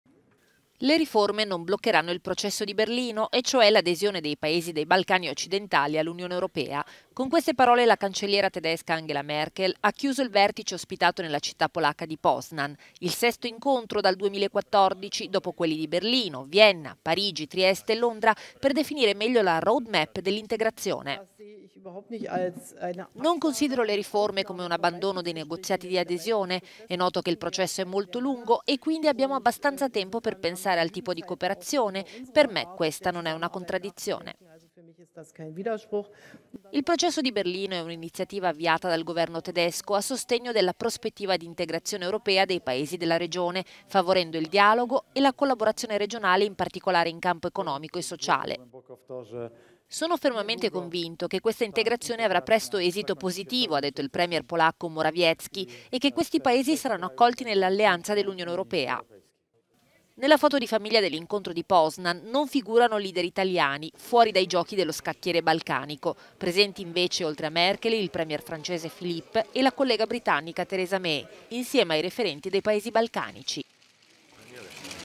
Balcani: vertice per l'integrazione con l'Ue
Merkel: "Processo lungo ma avviato".…